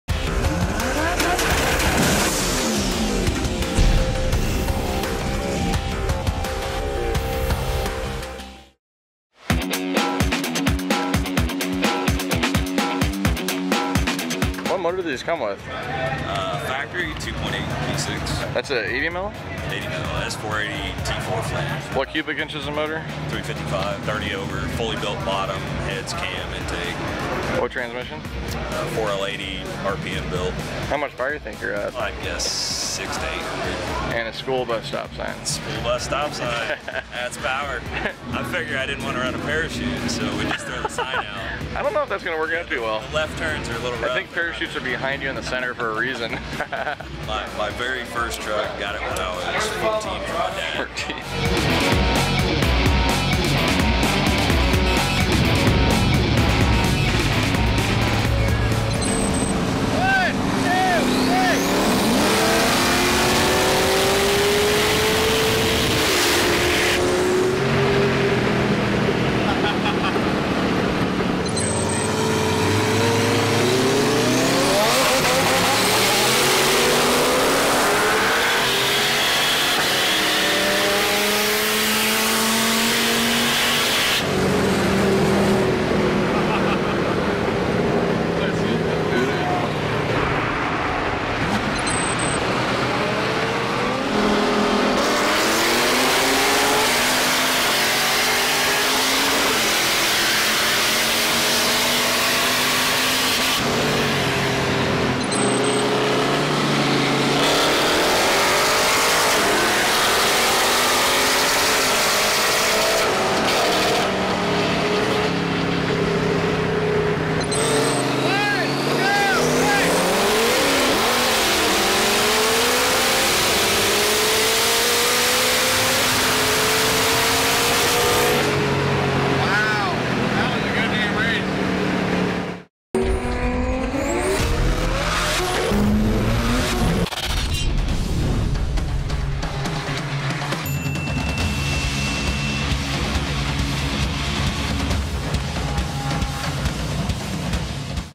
SPOOLBUS Goes Street Racing!
No one builds LT1’s anymore?  WRONG!  The Spool Bus is a mean turbo’d fire-breathing machine! Making over 700hp with an old LT1 and a giant 80mm turbo, this monster takes down a C7 Z06, big turbo Honda Civic, C6 Vette, C5 Vette, and many more! The school bus stop sign is the icing on cake, this build is one of the most unique things we’ve seen on the street! We love the Spoolbus and had a blast screaming down the streets!